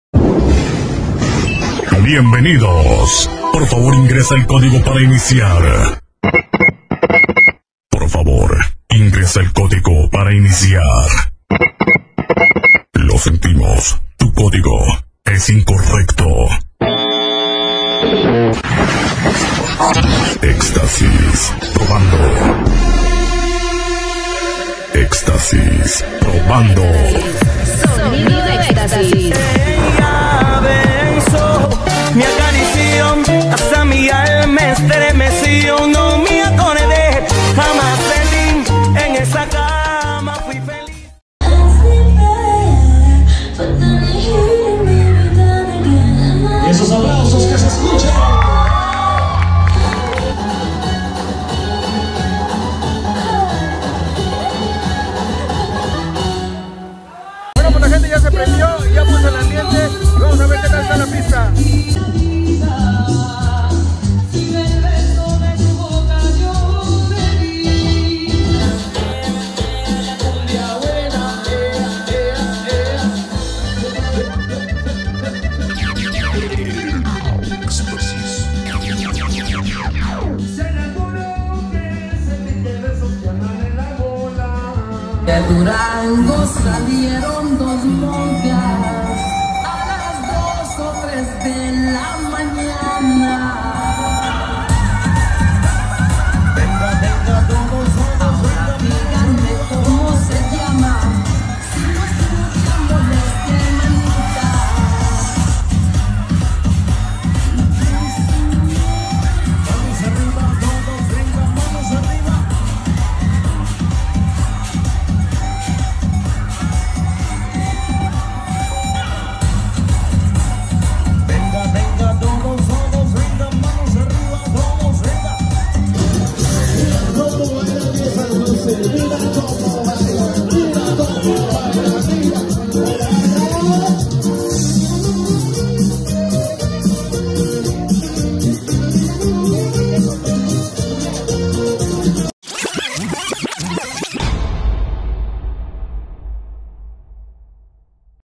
Dj Xtasis en Alcholoya Acatlán producción 23-24
Así nos presentamos en Alcholoya Acatlán en los XVI años de Lineth Analy